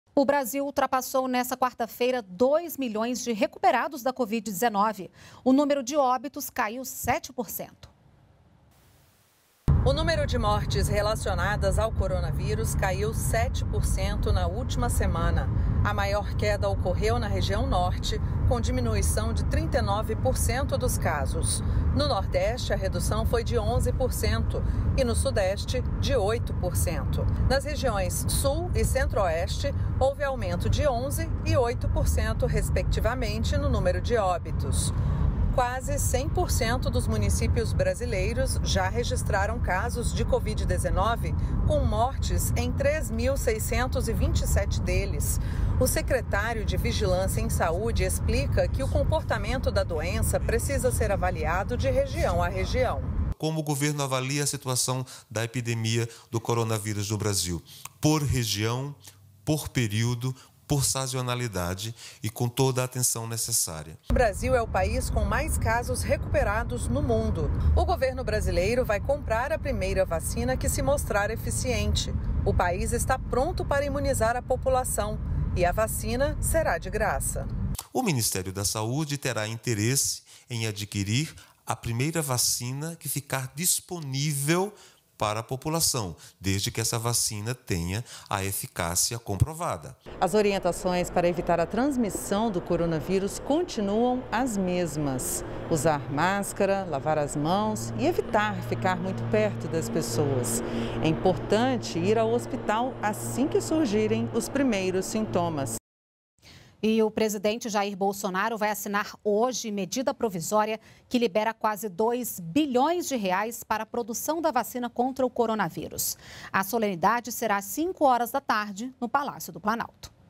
Ministério atualiza dados da Covid-19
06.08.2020 - O Brasil ultrapassou nessa quarta-feira dois milhões de recuperados da Covid-19. O número de óbitos caiu 7%. Foram registradas 97.256 mortes.